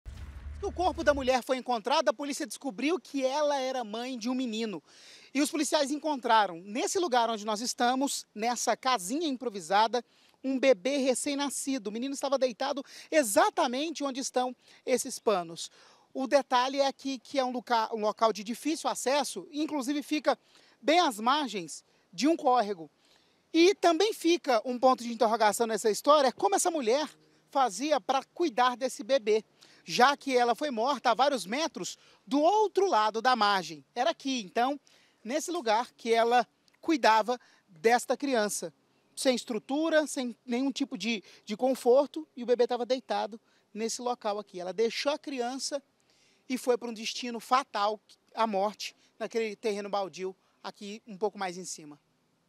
DUAS PESSOAS PRESAS: MULHER ASSASSINADA E BEBÊ ENCONTRADO ABANDONADO
CRIME MISTERIOSO! UMA DISCUSSÃO SERIA O MOTIVO DA MORTE DE UMA MULHER NO SETOR CRIMÉIA OESTE, EM GOIÂNIA. OS SUSPEITOS DE COMETER O CRIME, UM HOMEM E UMA MULHER, FORAM PRESOS. UM BEBÊ QUE PODE SER FILHO DA VÍTIMA FOI ENCONTRADO ABANDONADO ALGUNS METROS DE DISTÂNCIA DE ONDE TUDO ACONTECEU.